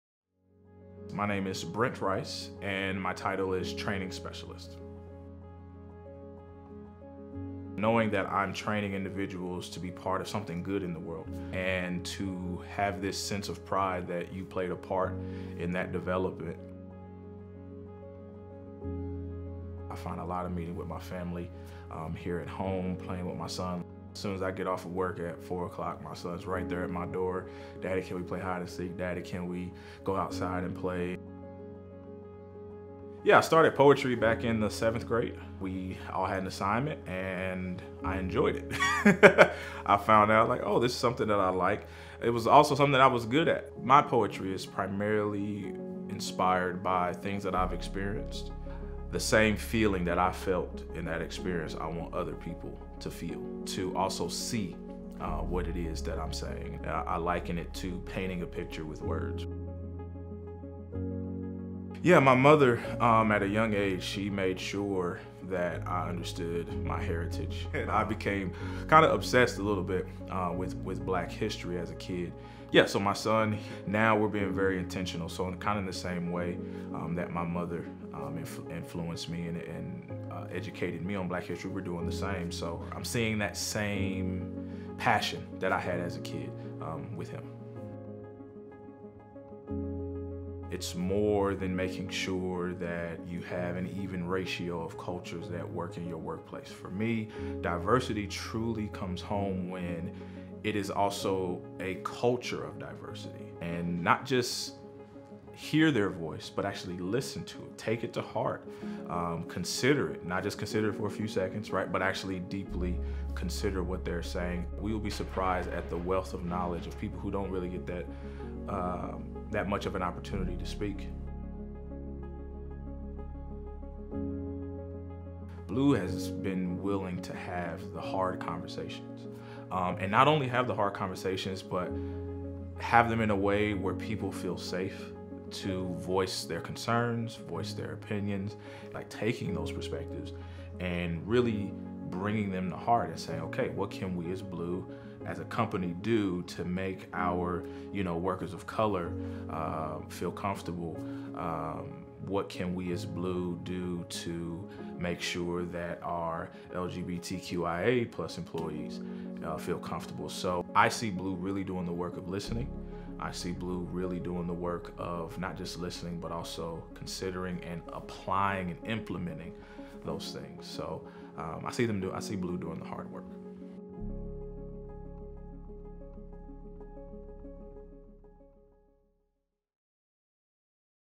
Employee Spotlight: Brent Rice, Training Specialist
Blue KC Training Specialist Brent Rice learned about Black history at an early age from his mother. Now, he’s instilling the same knowledge and values in his own son. One of the ways Brent expresses himself, his experiences and his heritage is through poetry.